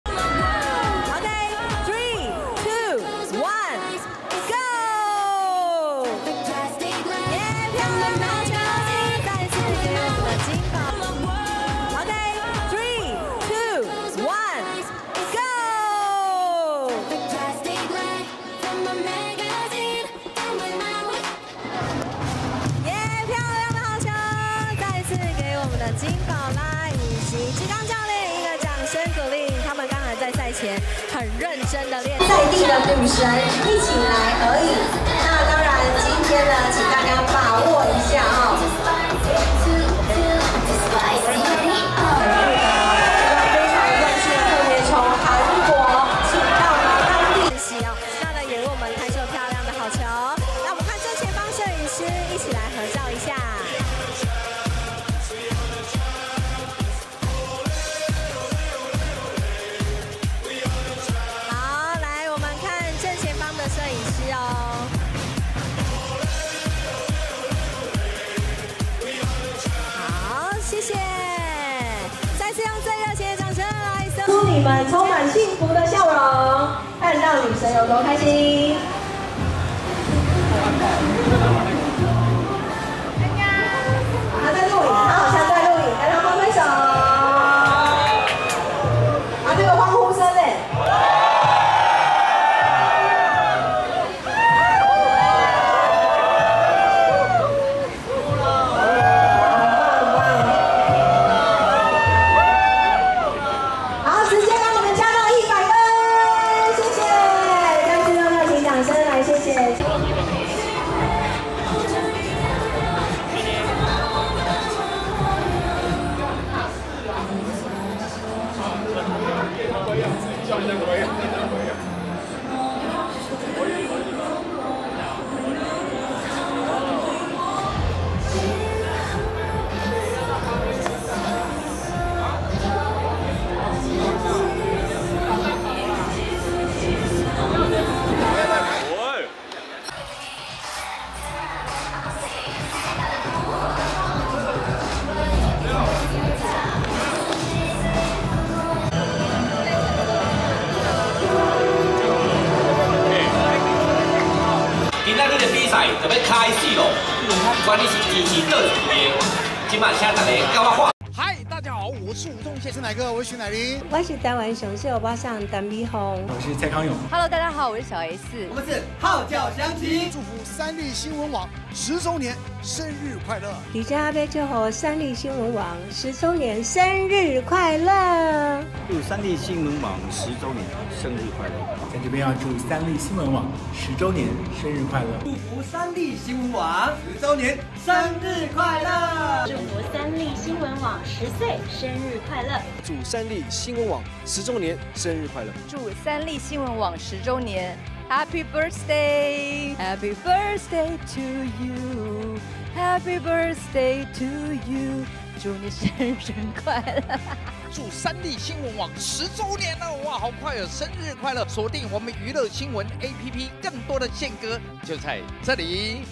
獨家／金寶拉參加統一獅女神日！女神自曝想演台八｜三立娛樂星聞
來源:YT/CPBL 中華職棒
金寶拉提供